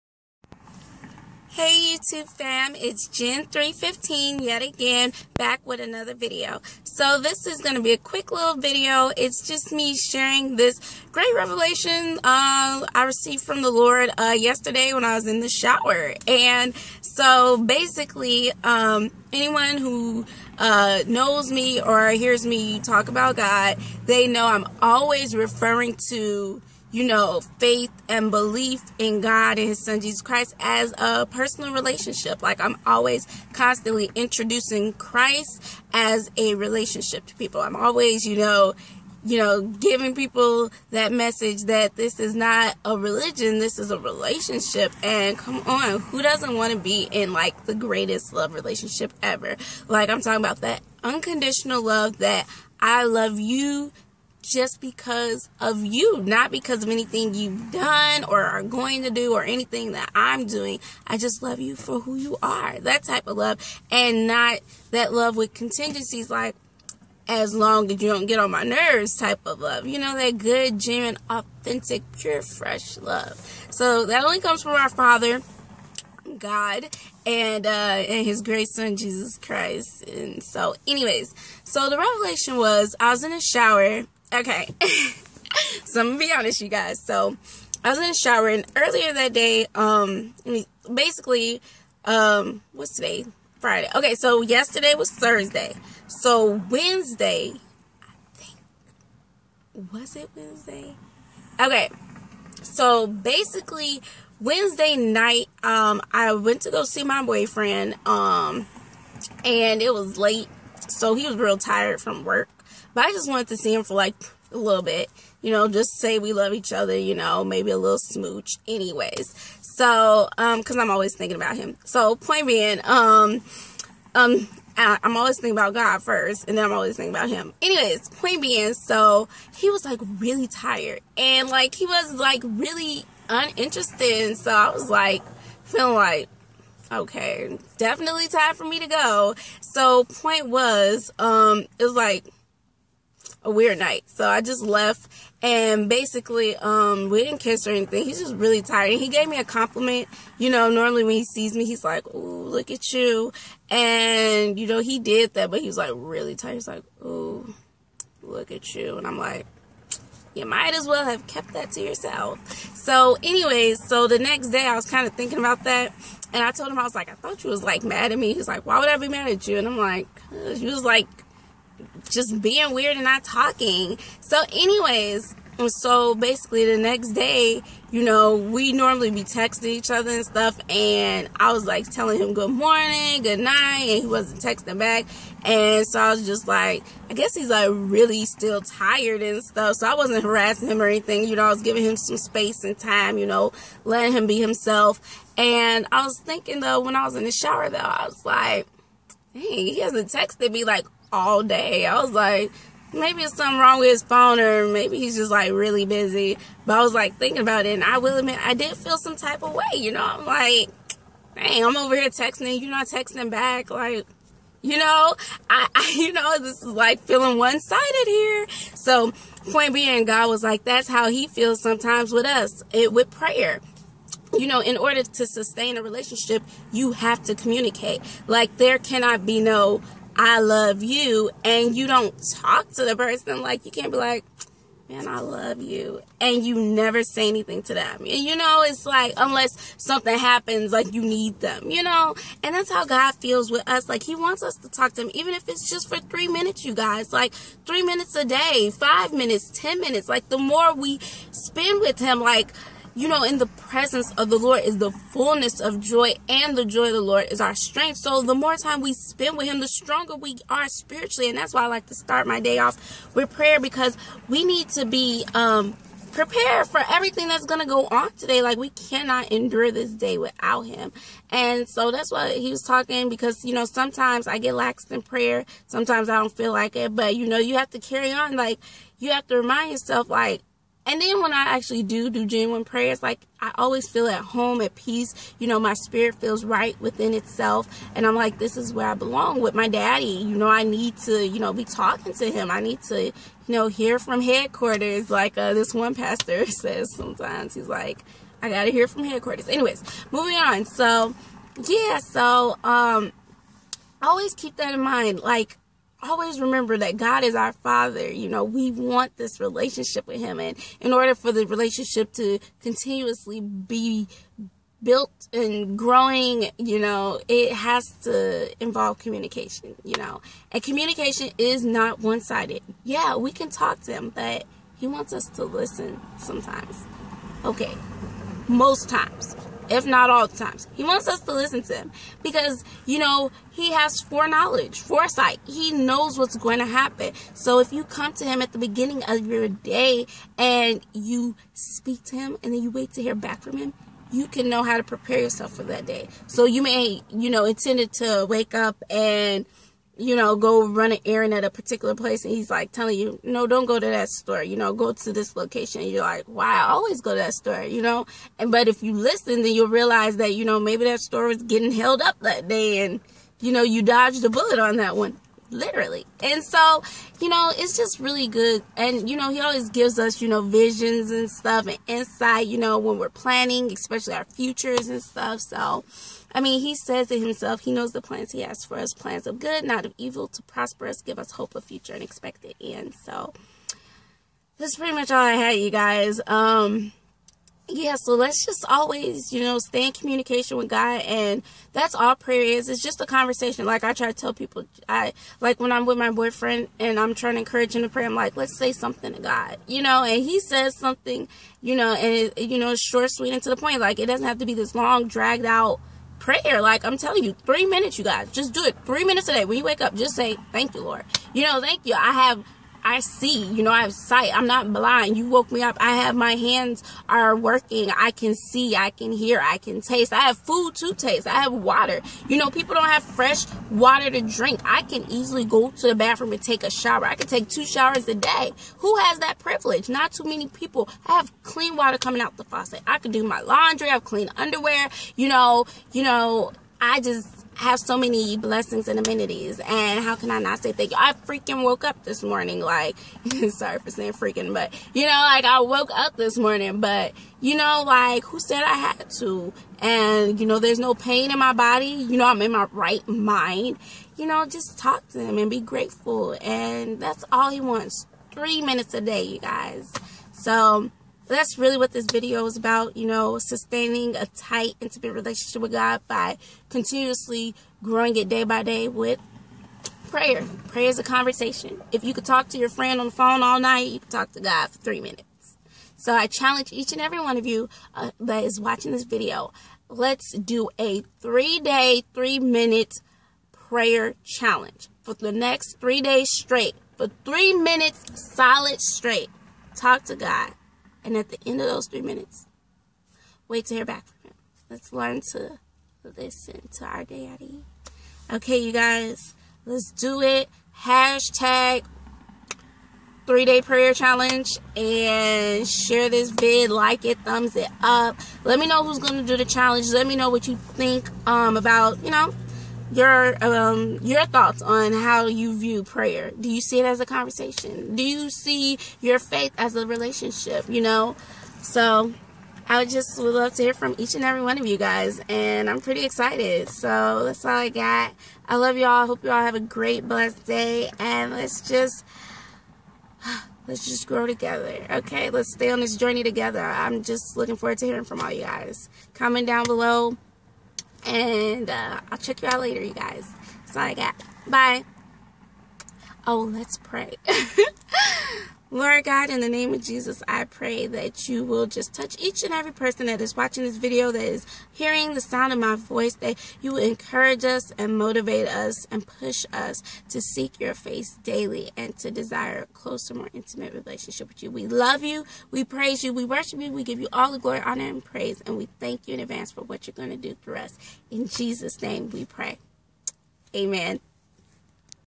PRAYER - 3 Day Challenge!
In order to sustain and develop a relationship, the parties involved need to regularly communicate. Our relationship with God our Father calls for us to grow daily through Him by means of a little two-way conversation called prayer. Our Daddy waits daily to hear from us, let's talk to Him. I challenge each of you to join me in a three day challenge where we pray to God for a minimum of 3 minutes straight. (if your average prayer life extends 3mins, then add an additional 3 to what you normally do) Like and share this vid, for all who are taking this challenge, and I look forward to hearing how God is transforming your lives 😊! ttys...